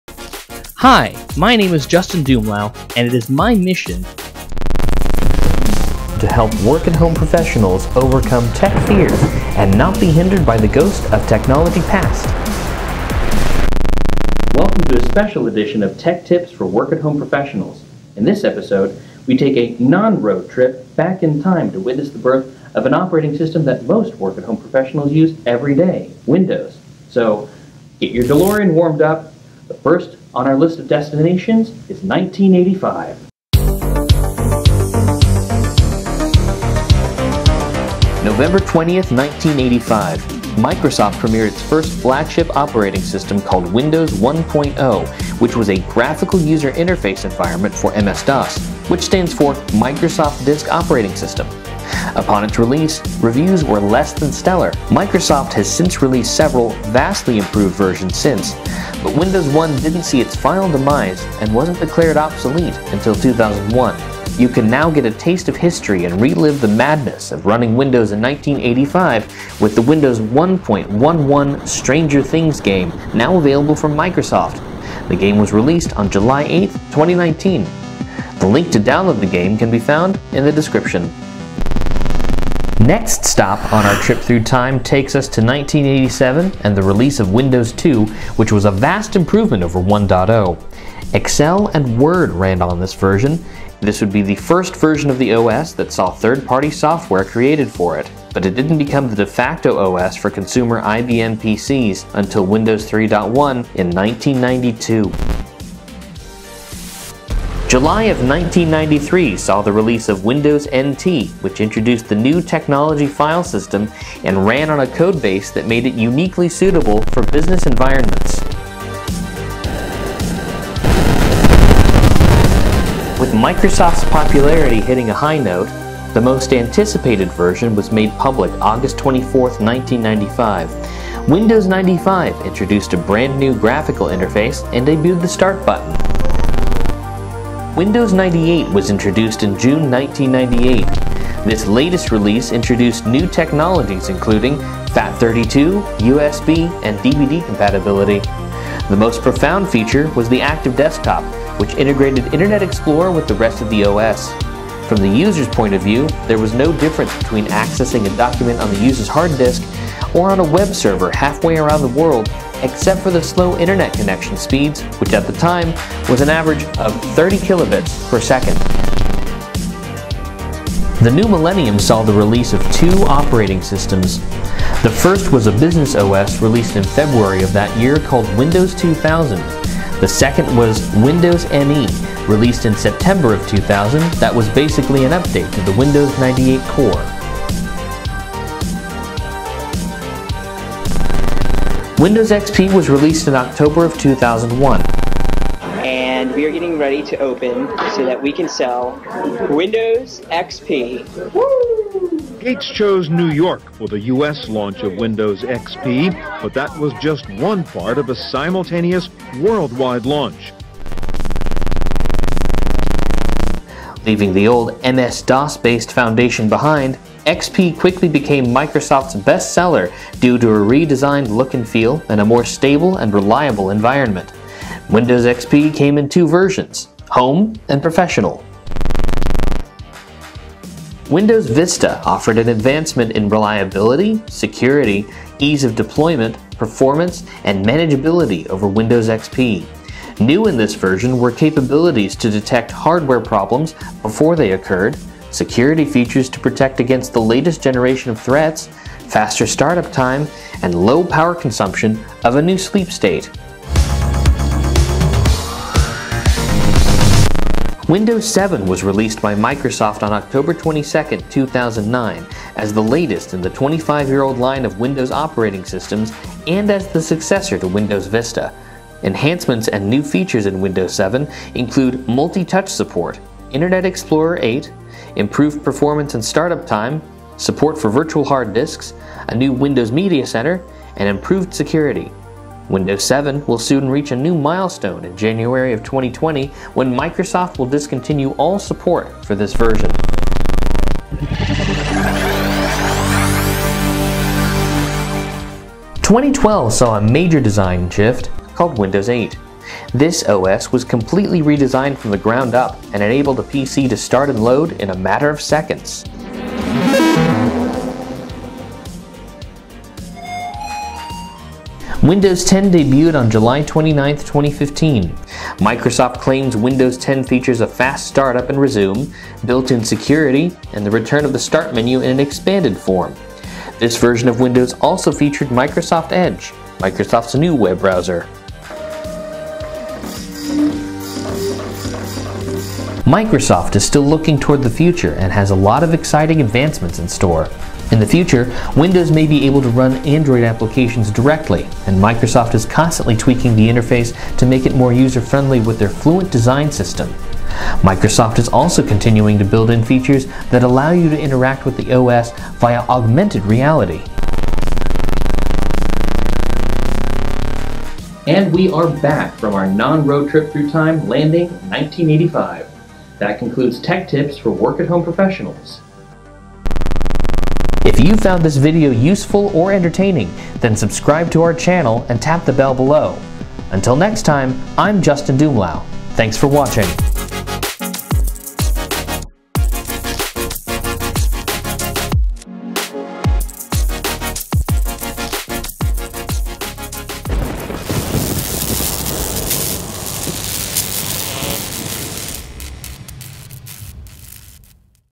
The history of Microsoft Windows inspired by Stranger Things 3 from 1985 to 2019.
Inspired by Stranger Things 3, we decided to travel back and experience the evolution of an operating system called Windows. Microsoft released Windows 1.0 November 20th, 1985. Since then the operating system has gone through a massive evolution over the past 34 years. We take a time machine from 1985 through 2019 and a glimpse into the future of Microsoft Windows.